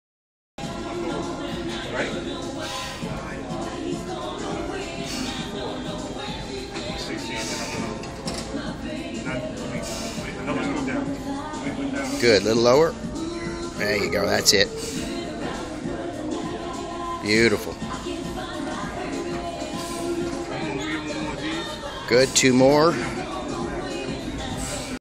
One arm Incline Dumbbell Bench Press
How to properly execute a 1 arm incline dumbbell bench press